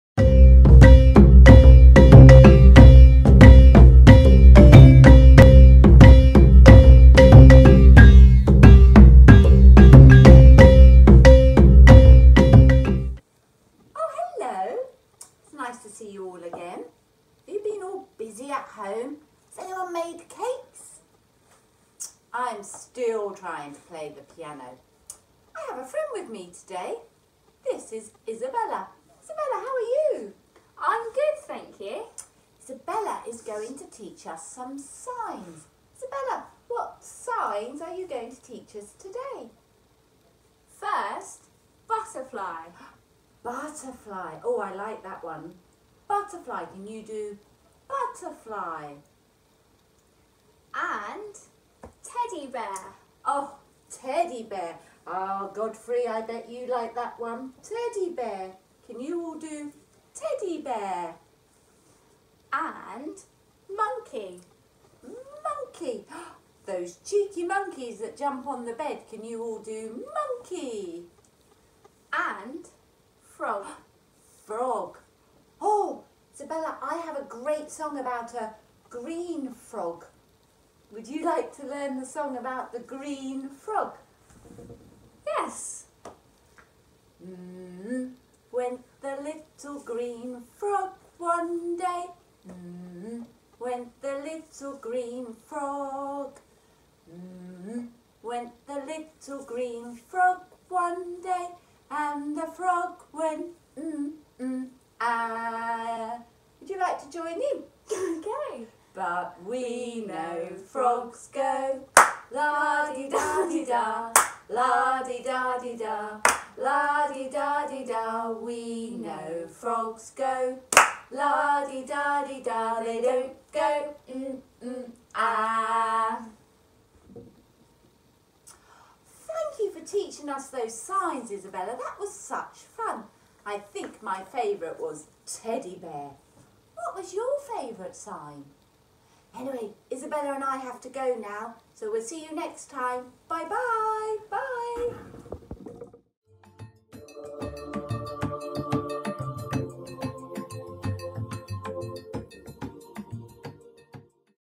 Let's have fun signing! - PAWS Nursery
Some fun signs and a song for all the children at home.